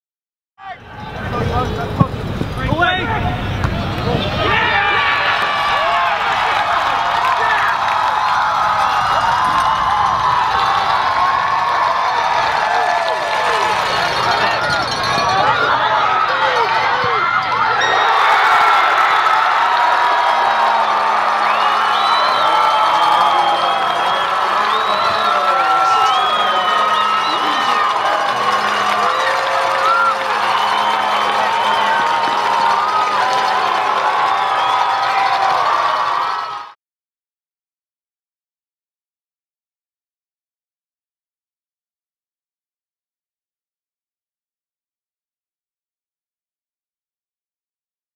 M-SOC: Sheldon, Palmer Propel Aggies to Tournament Final
UC Davis sophomores Matt Sheldon and Ian Palmer each found the back of the net in the Aggies' 2012 Big West Tournament semifinal match to earn their squad a berth in the Big West Tournament final.  UC Davis defeated Cal State Fullerton 2-0 to advance to their first tournament title match.

2012 Big West Men's Soccer Tournament semifinals, (N1) UC Davis vs. (S2) Cal State Fullerton November 7, 2012 at Aggie Soccer Stadium on the campus of UC Davis.

Footage courtesy Greg Fulks.